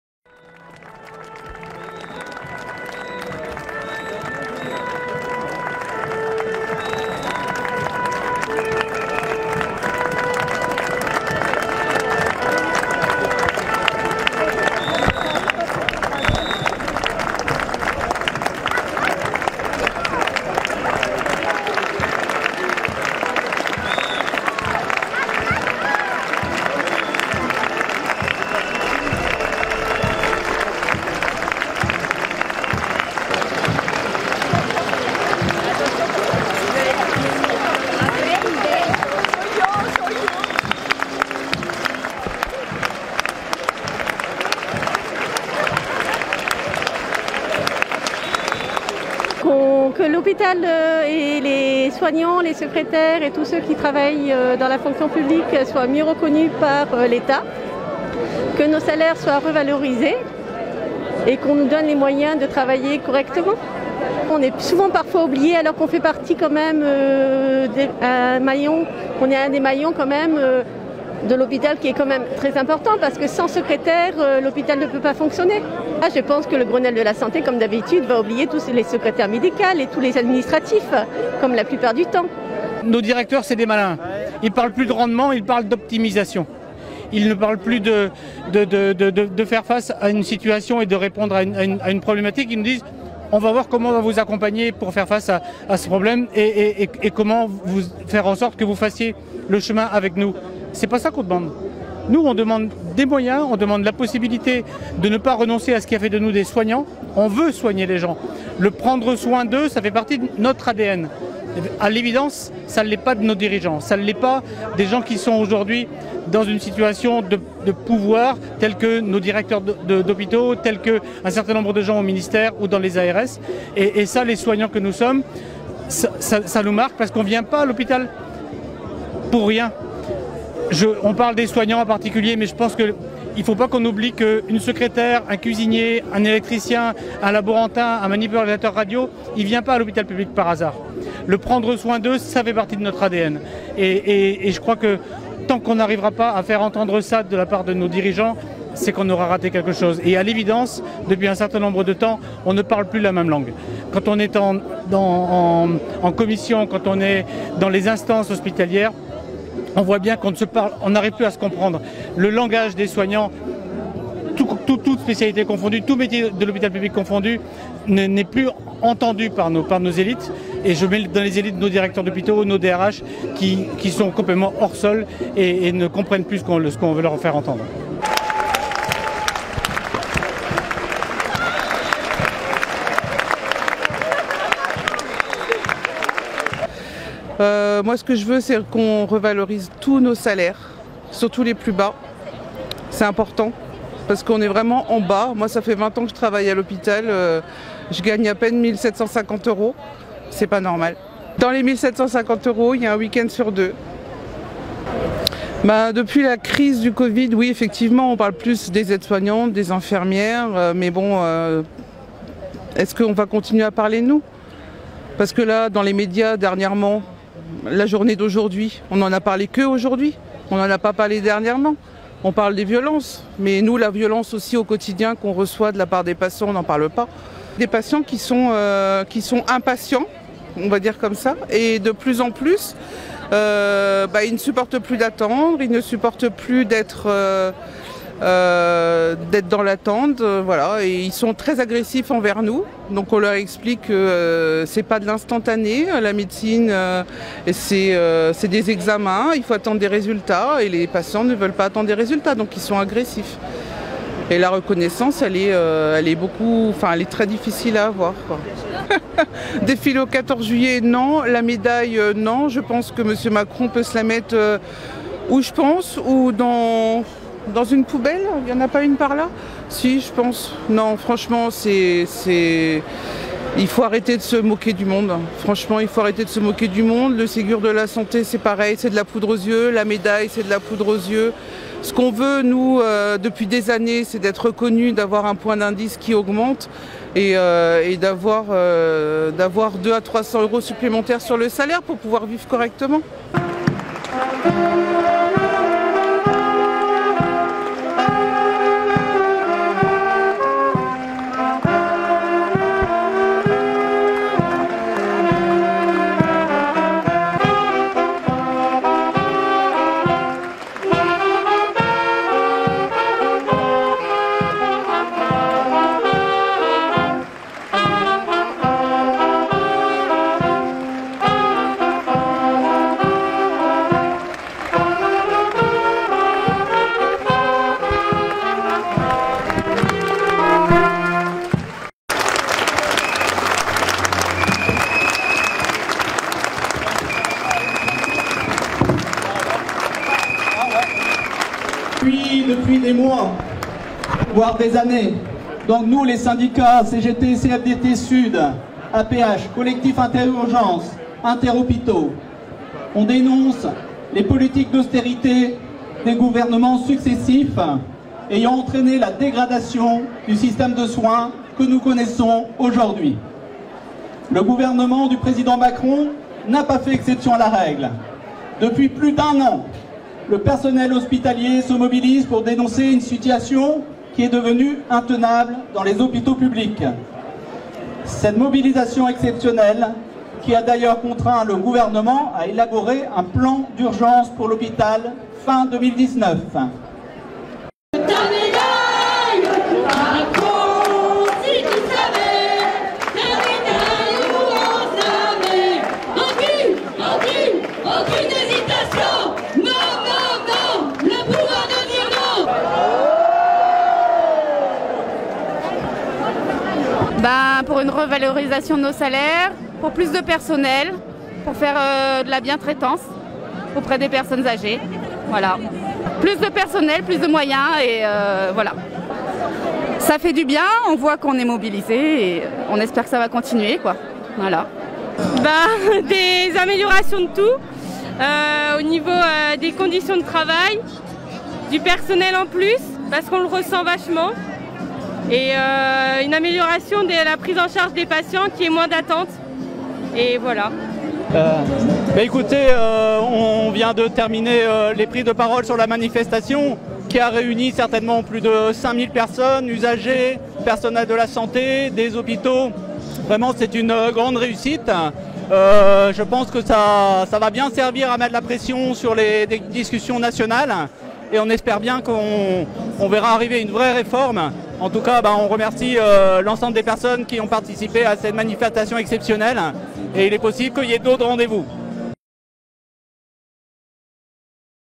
Mobilisation historique au CHRU Brest-Carhaix le 16 juin 2020
Mobilisation historique au CHRU Brest-Carhaix le 16 juin 2020 pour la défense de l'hôpital public et du système de santé.